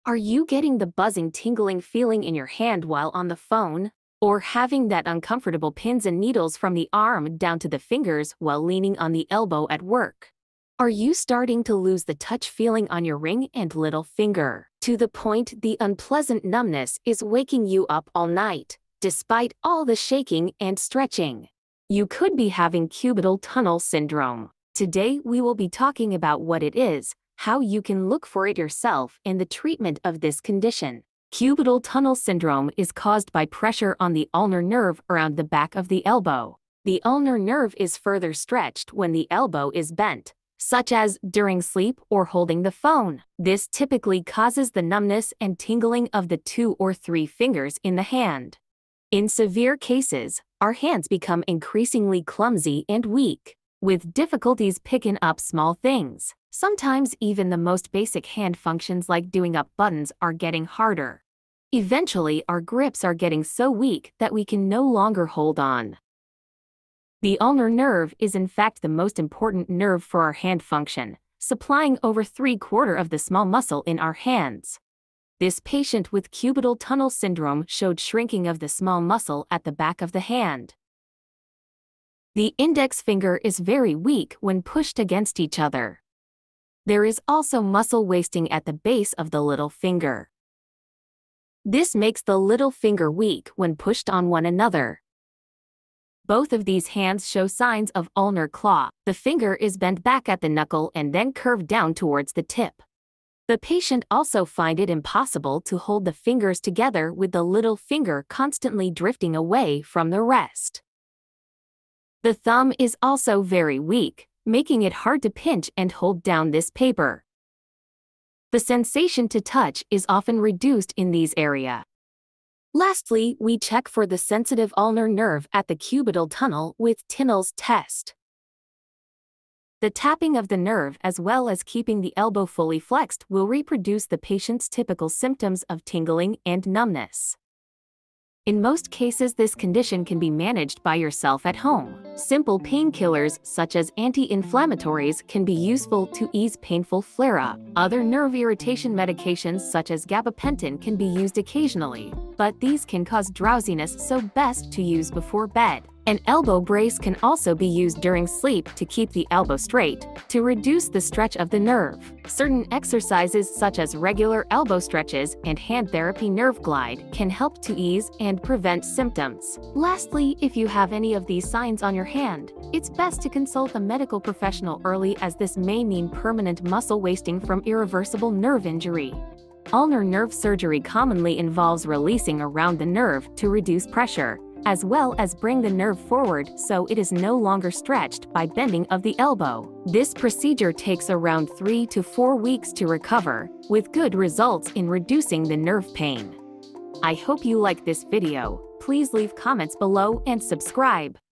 Hand pain and numbness: Cubital tunnel syndrome
Learn about Cubital tunnel and ulnar nerve problem in 4 minutes
cubital tunnel elbow brace:

Thanks to @3D4Medical